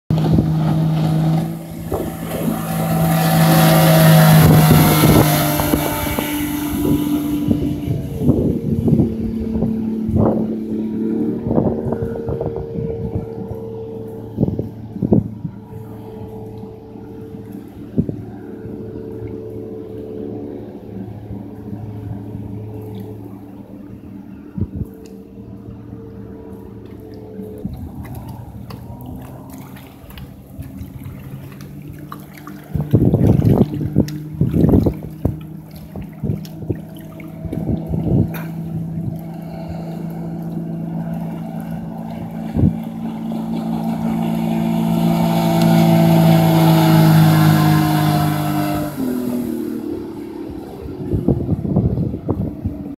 Champion 20' Super V with Mercury 200hp EFI
Running at 60mph GPS on Leaf Lake, Lake Park, MN. Sept. 30th, 2015